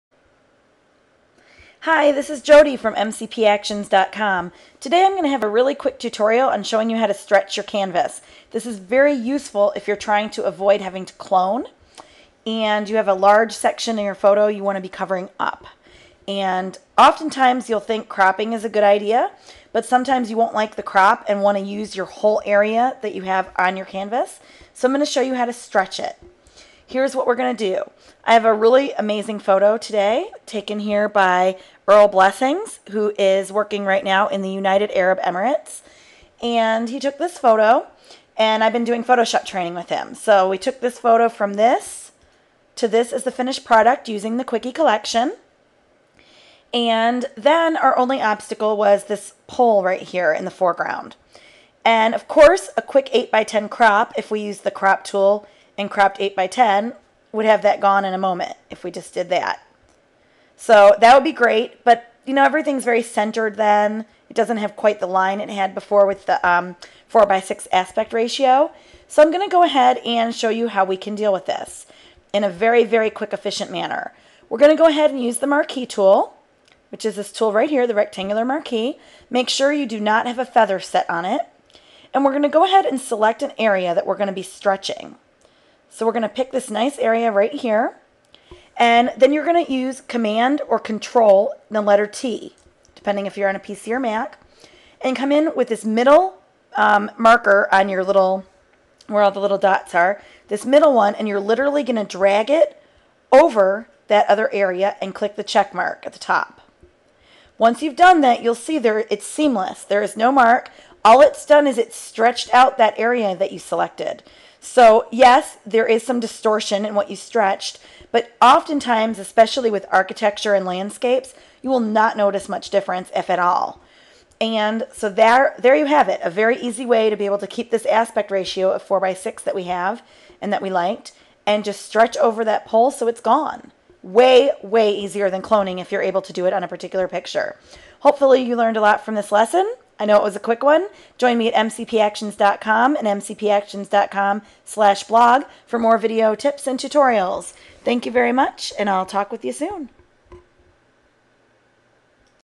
Learn to Stretch Your Canvas in Photoshop
Did you run out of room - which you have more space on your photo?  Do you wish you could extend the scene more?  This tutorial will teach you how to Stretch your Canvas in Photoshop. This is a great tool and sometimes can save you hours instead of having to use the clone tool!

for more Photoshop tutorials and great tools to make your life as a photographer easier.